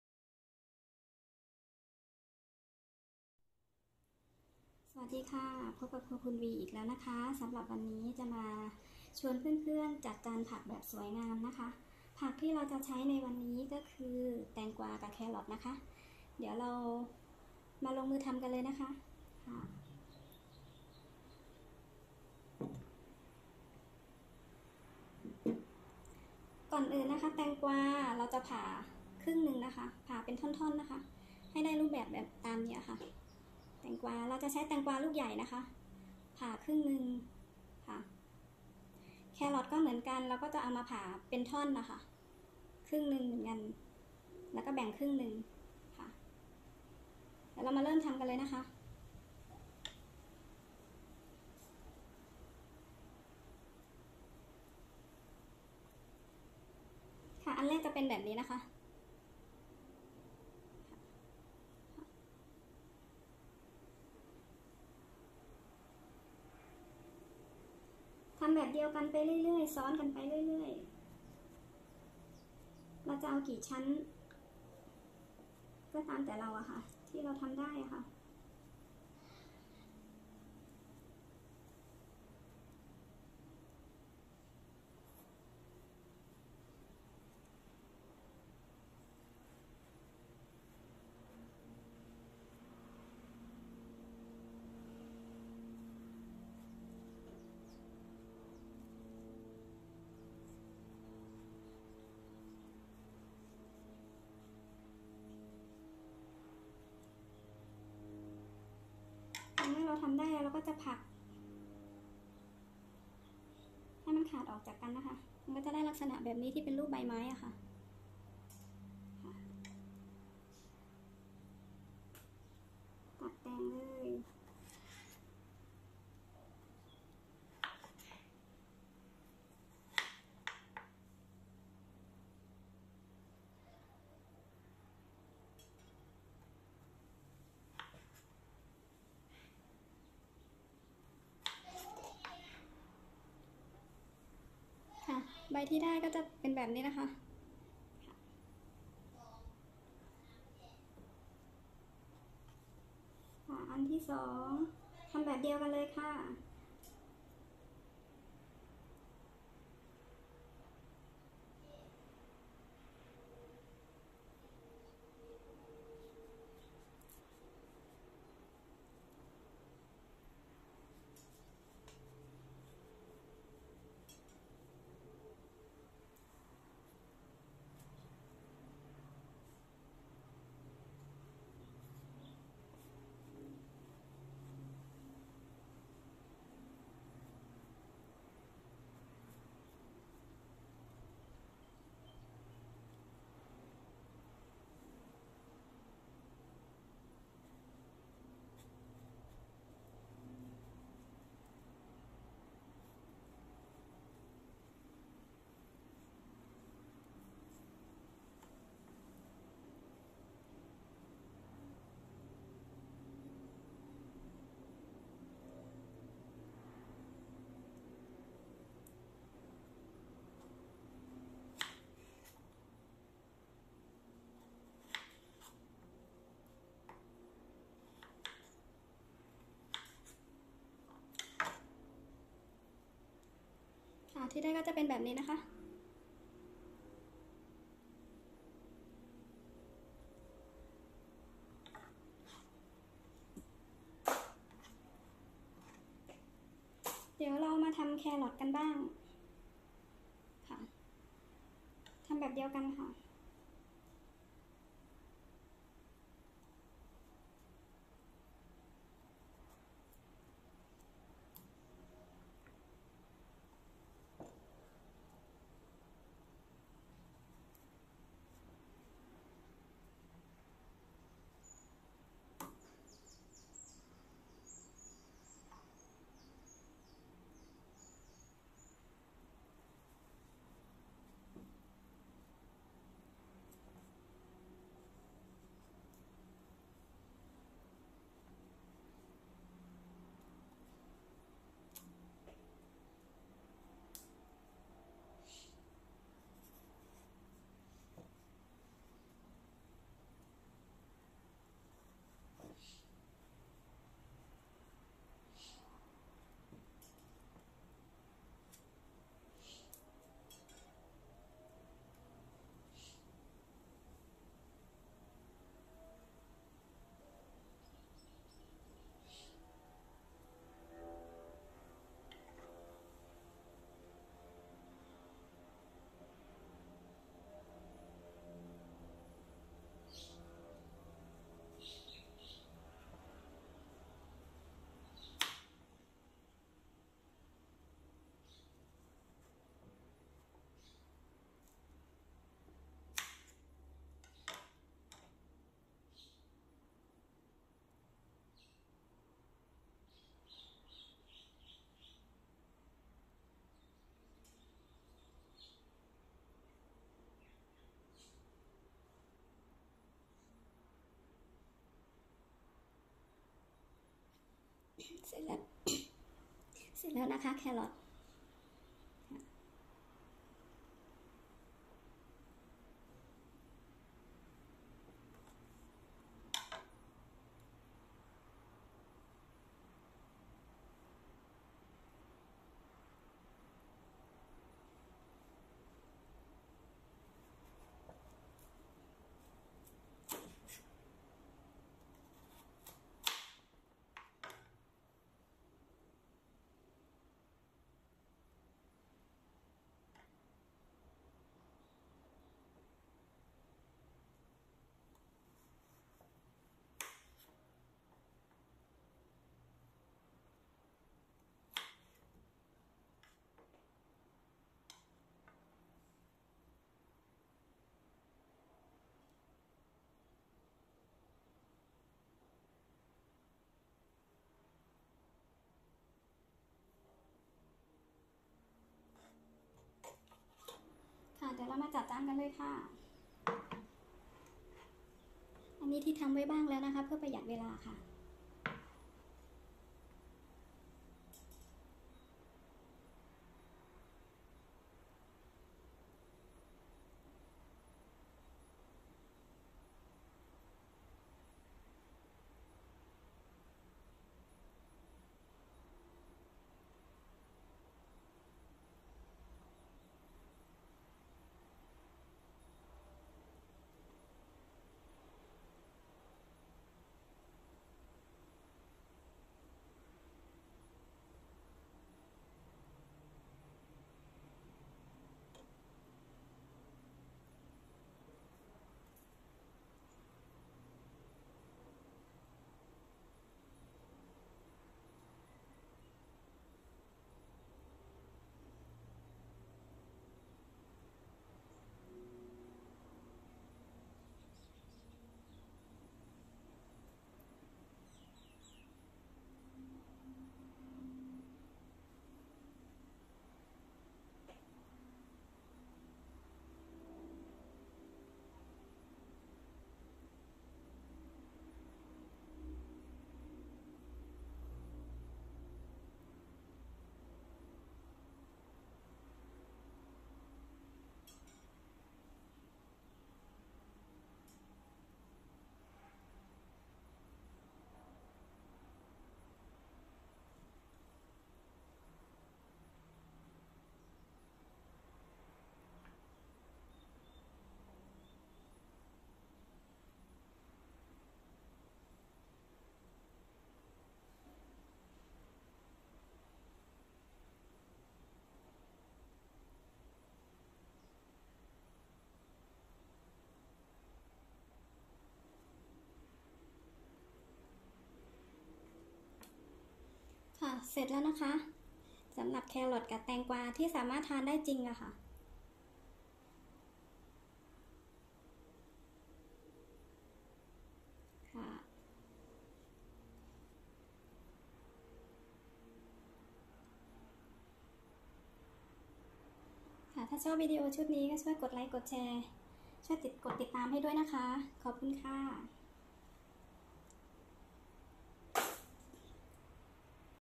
วิธีจัดจานผักแบบสวยงามอย่างมืออาชีพ​ ไม่ใช่มืออาชีพคุณก็ทำได้
แตงกวาควรใช้ลูกใหญ่ส่วนแครอทปอกเปลือกก่อนถ้าไม่เอาเปลือกออกจะดำ